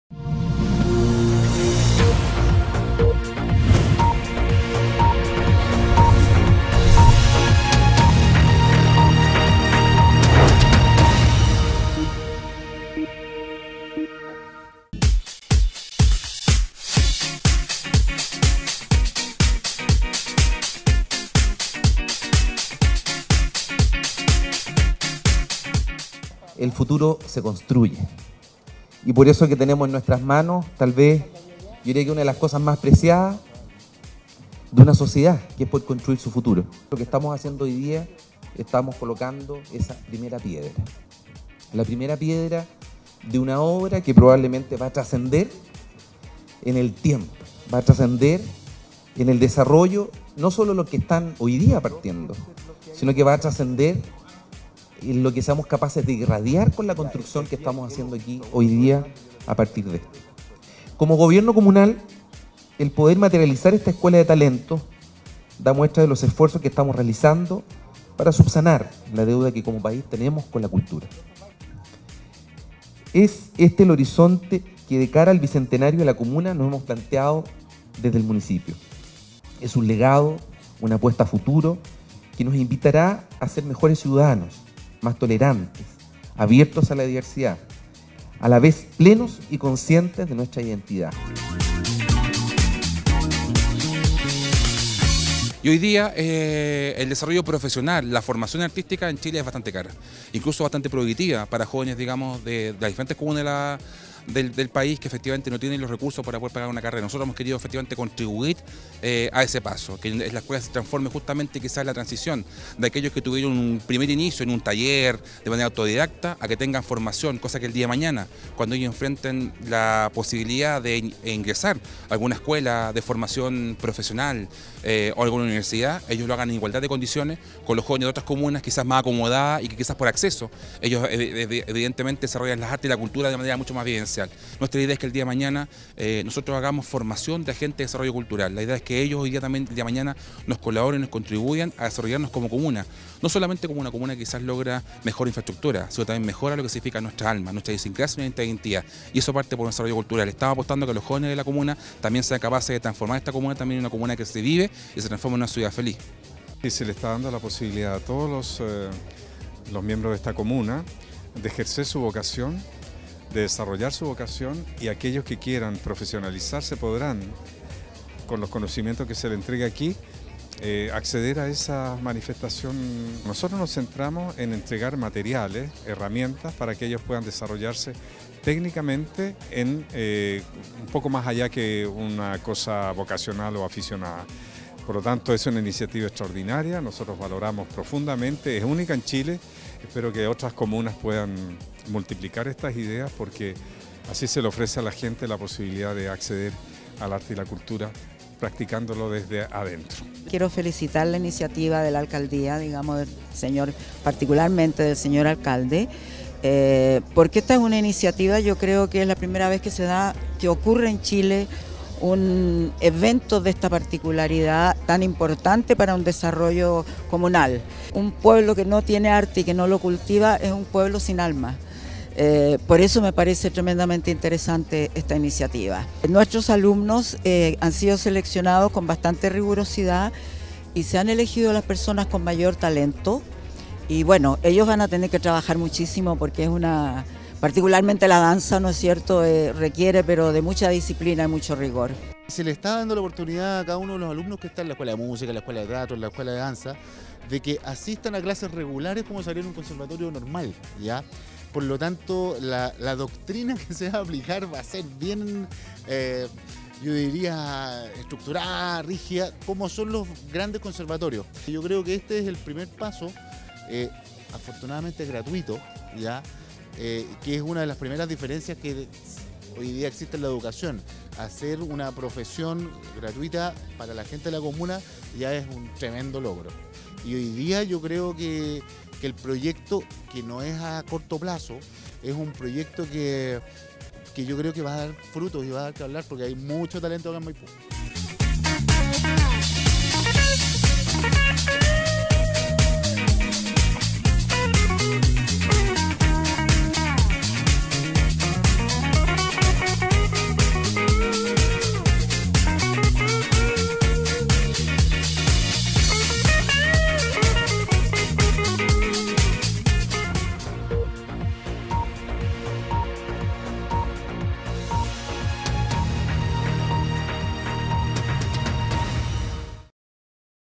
MAIPÚ LO LOGRA:CULTURA GRATUITA Y DE CALIDAD
En el Teatro Municipal de Maipú se realizó la inauguración de la Escuela Superior de las Artes de Maipú (ESAM), donde reconocidos profesionales dirigirán a los alumnos que desean avanzar en el camino de las artes para luego profesionalizarse en diversas áreas.

Carlos Figueroa, Alejandro Castillo y Magali Rivano, son quienes dirigirán las diversas áreas de "ESAM"- música; actuación; y ballet, respectivamente- para que los alumnos puedan conocer el desarrollo de sus aficiones de manera más profesional. Todo esto, gracias a la gestión del Alcalde Christian Vittori, es absolutamente gratuito.